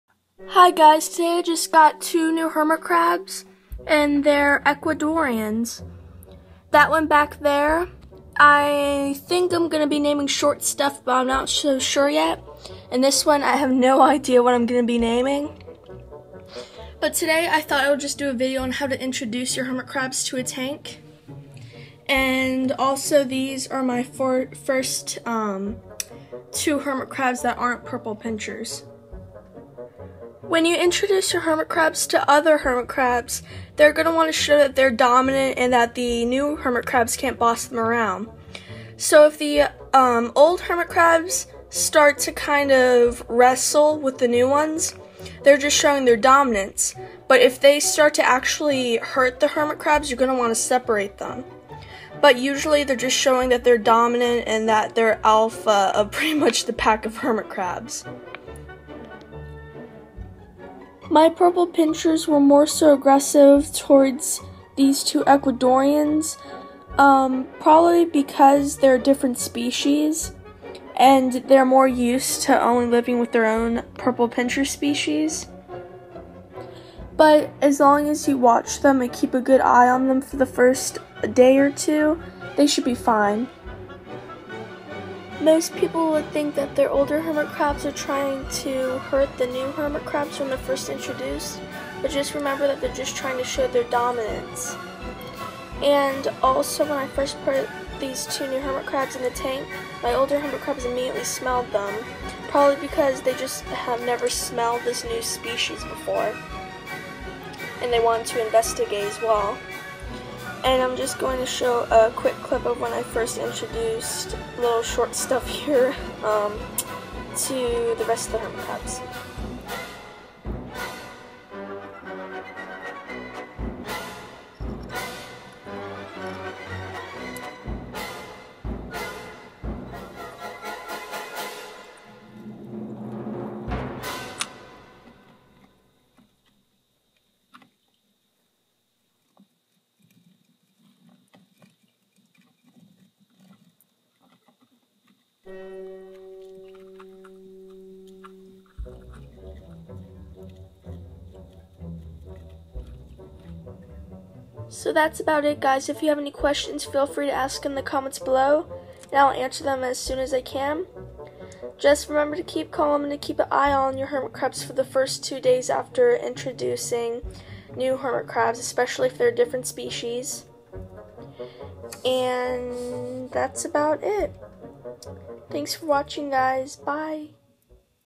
Basic Hermit Crab Care: Part 3 - Introducing New Crabbies
In this video of "Basic Hermit Crab Care", I'll show you how and what to expect when introducing new hermit crabs to your tank!